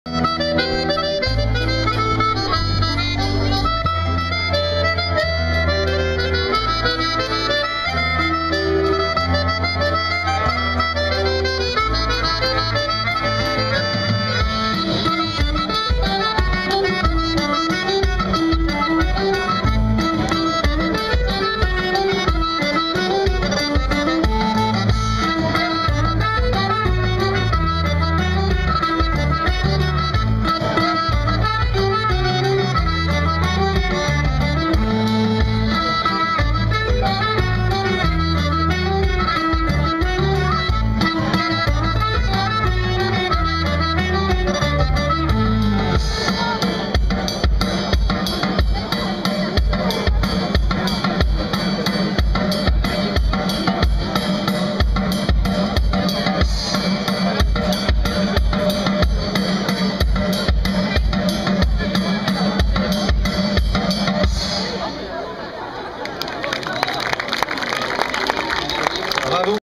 MoTa / Kas Ha Barh au Folies Tapping de Trentemoult
MoTa au fest deiz des folies tapping de Trentemoult.
Kas ha Barh.
Reze Trentemoult, mai 2012.